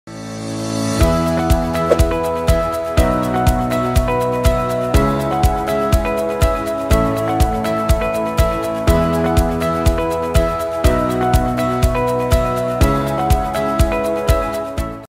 Frozen Goods Delivered Quickly from Colon | Just Lalamove It | Lalamove Cebu
Keep frozen products fresh from your supplier's freezer to yours 😋
Quickly deliver your products with a Motorcycle delivery from Colon!